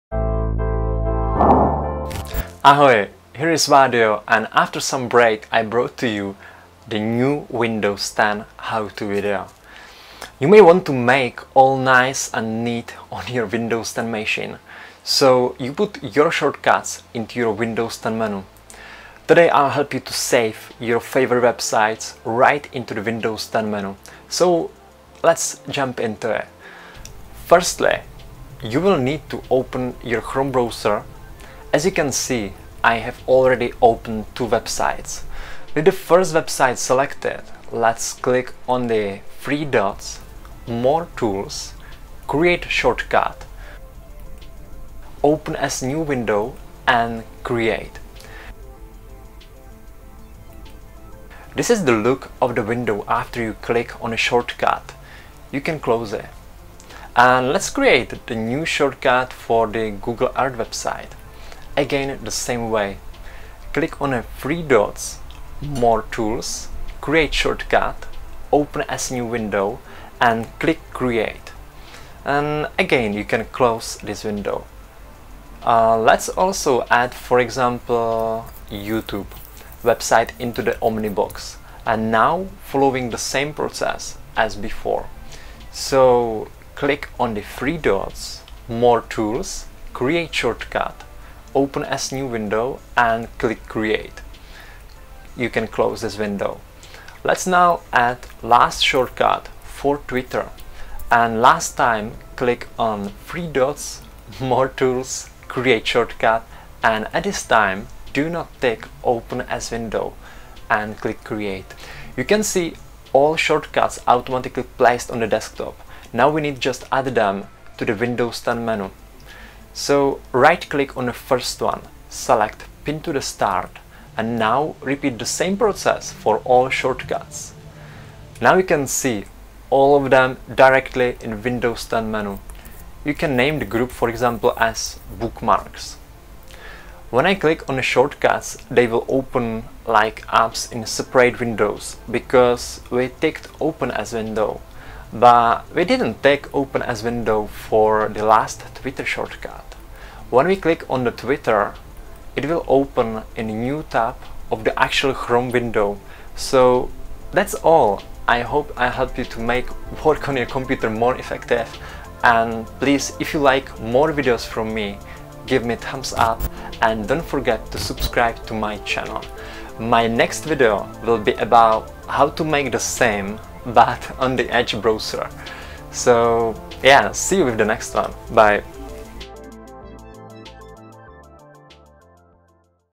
How to add Chrome Bookmark to Windows 10 menu
A short guide on how to add bookmarks of your favourite websites directly into your Windows 10 menu to make your workflow on your Windows 10 more effective.